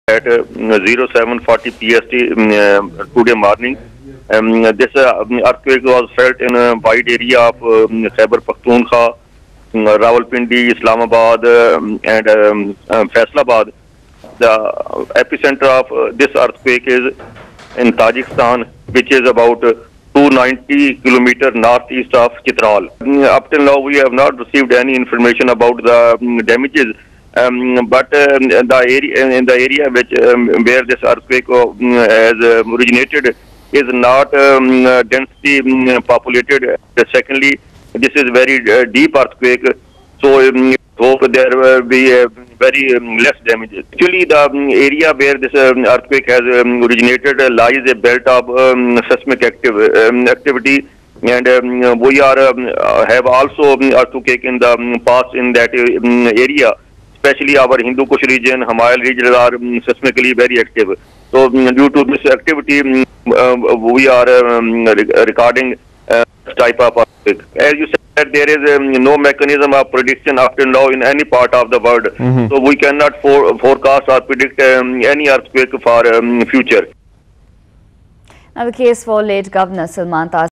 Earthquake jolts parts of Pakistan
A 6.3 magnitude earthquake hit different parts of the country early on Monday, including Islamabad, Rawalpindi and Peshawar, but there were no immediate reports of damages.